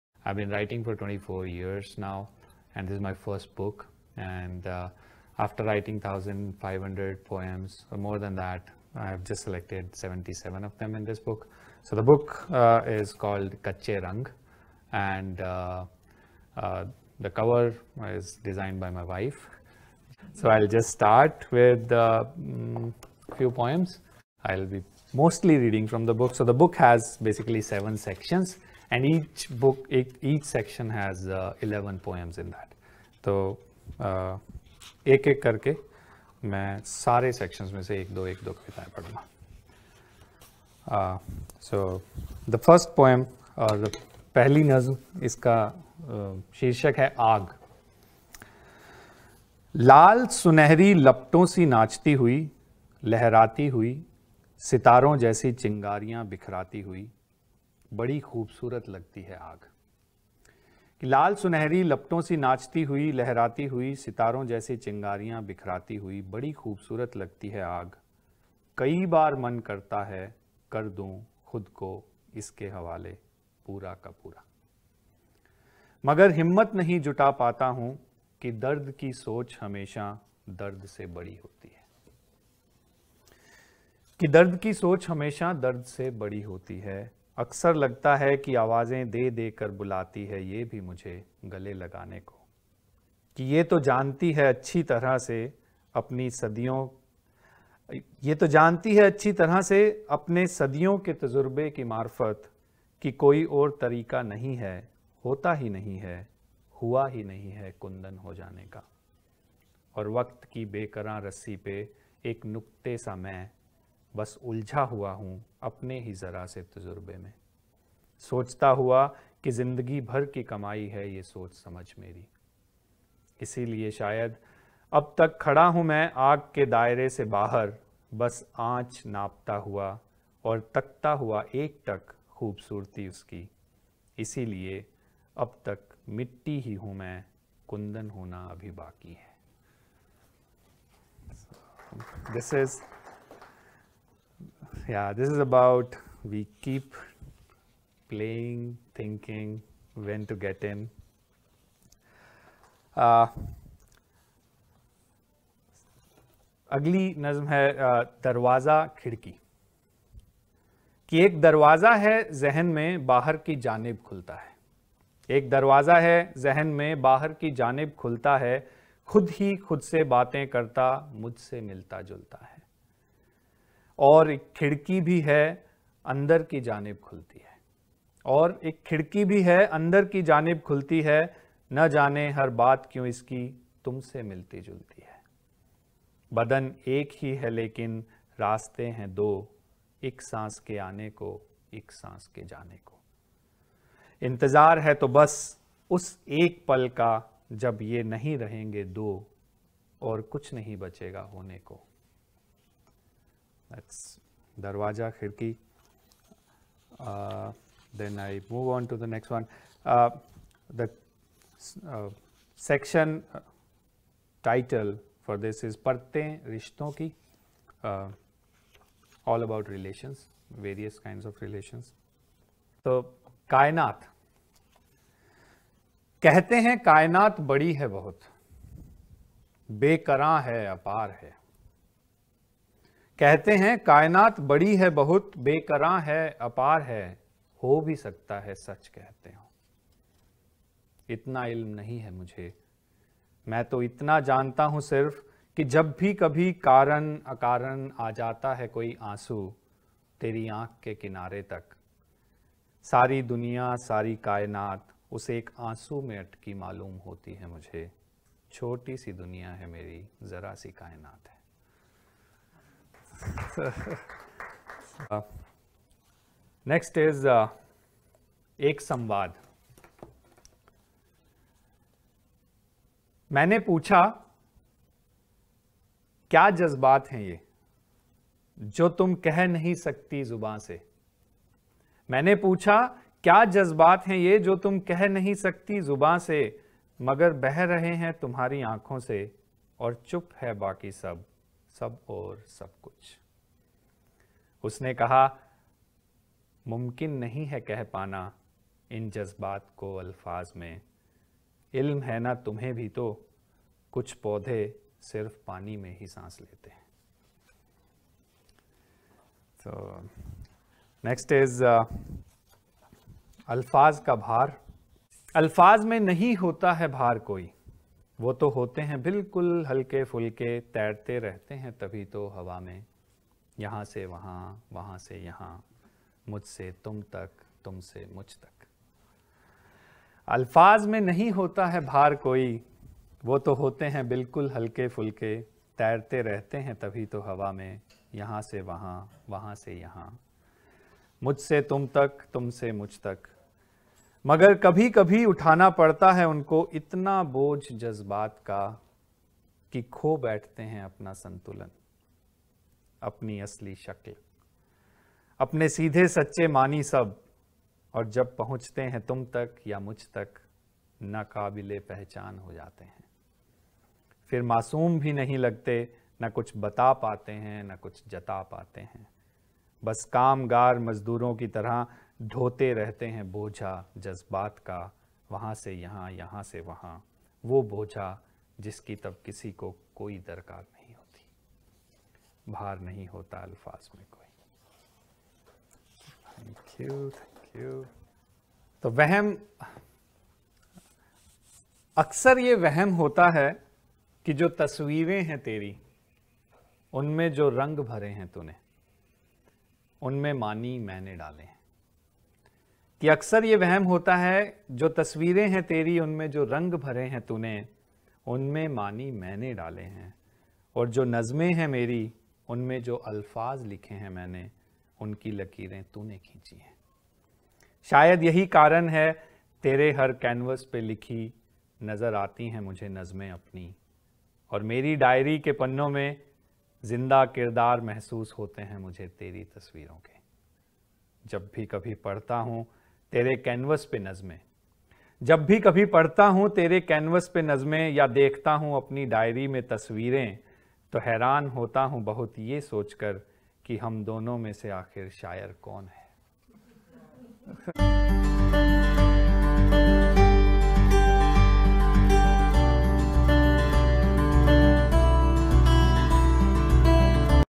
Kachche Rang - Palo Alto Reading - Part 1
Poet : Mohit Kataria
Cover Design : Anu Verma Kataria
Blurb : Gulzar Saab
Forward : Yatindra Mishra
Video: Shobana Swamy
Event Organized by : Rachna Dhir
Publisher : Vani Prakashan
Thanks to all the friends who came on a busy Saturday to attend the poetry reading.